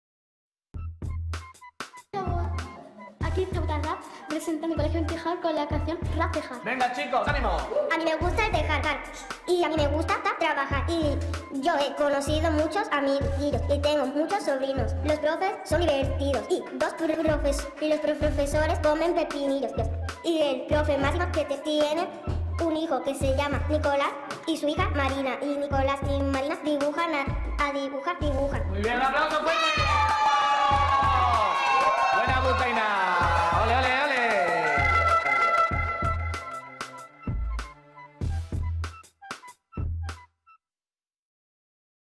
RAP TEJAR 2015 BOUTAINA + JAIRA 2º B 2014-2015
Los niños han creado sus propios rap. Tema libre a elegir entre varias bases musicales. Competencia linguística integral.